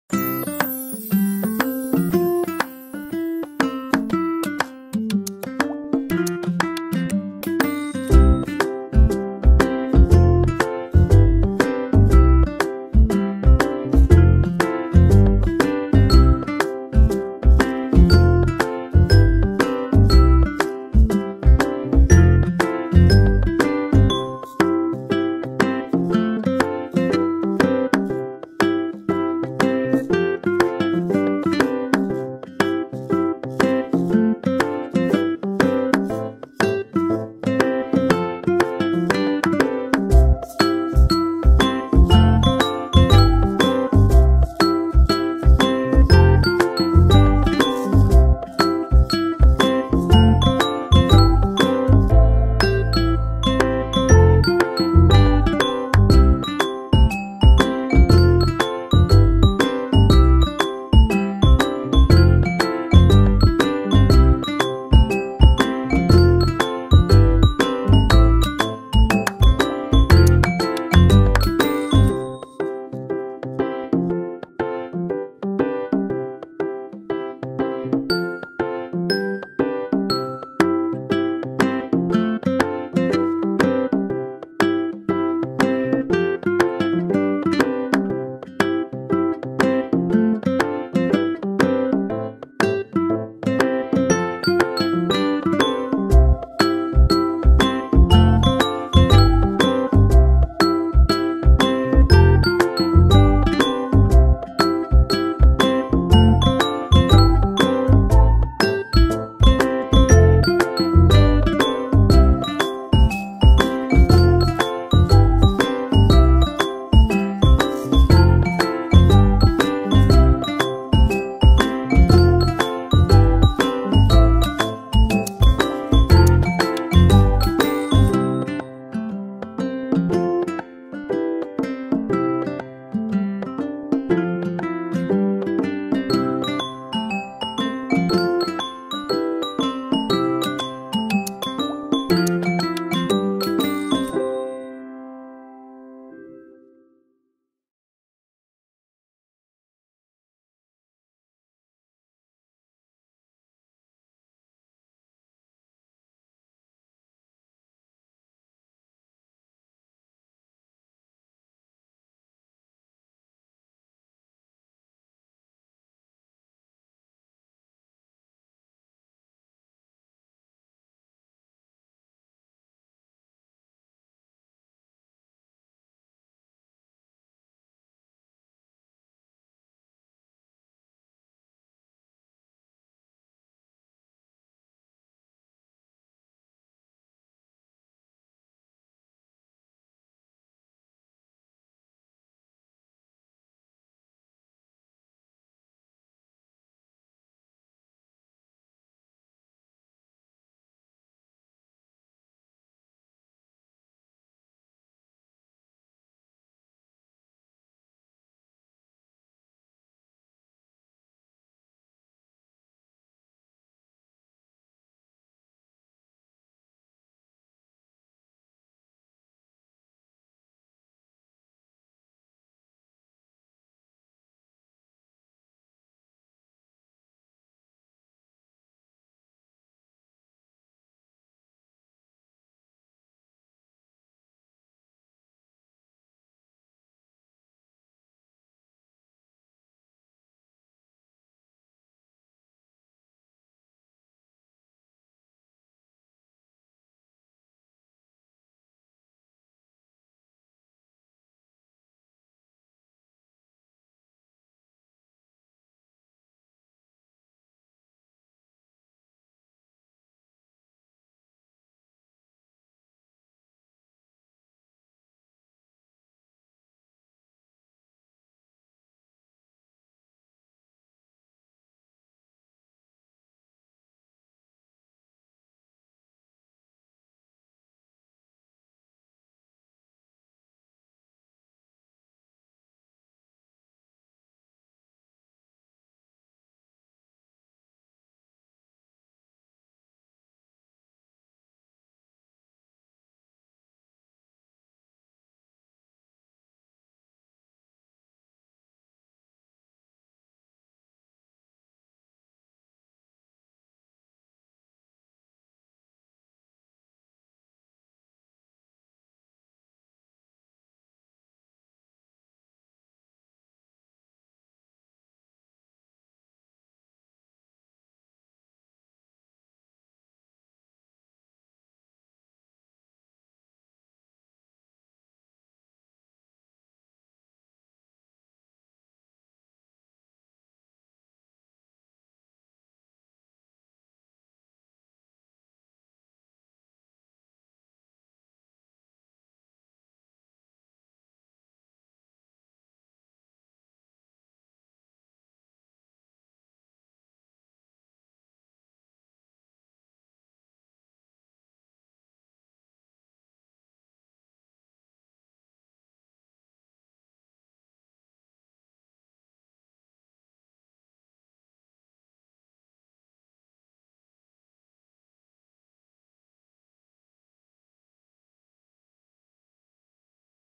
Small backyards with big gardens. Secrets and hacks into creating a beautiful garden for vegetables
Small backyards with big gardens. Secrets and hacks into creating a beautiful garden for vegetables, fruits, flowers and herbs.